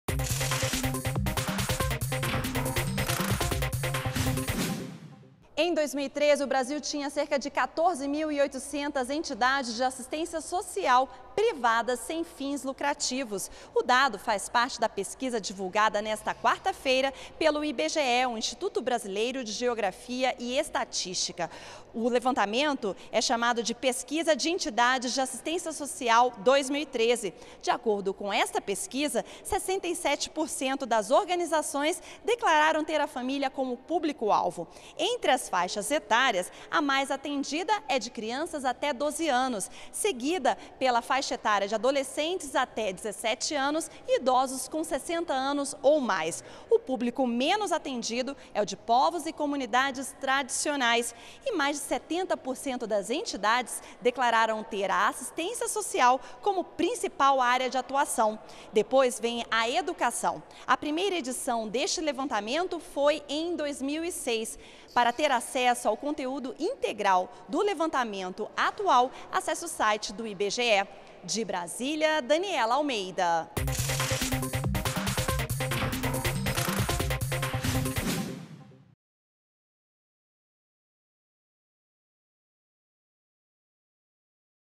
IBGE divulga pesquisa sobre entidades de assistência social sem fins lucrativos
ÚLTIMAS - 26.11.14: O IBGE divulgou nesta quarta-feira (26) a pesquisa de Entidades de Assistência Social Privadas sem Fins Lucrativos (PEAS) 2013. Existem atualmente em todo o país 14.791 de entidades do tipo e cerca de 60% delas tem a família como público-alvo. Segundo o levantamento, 72,6% das entidades também declararam ter a assistência social como sua principal área de atuação. A PEAS 2013 foi dividida em duas fases. Os dados apresentados fazem parte da primeira fase e examinam o público-alvo, a abrangência territorial e os serviços prestados pelas entidades.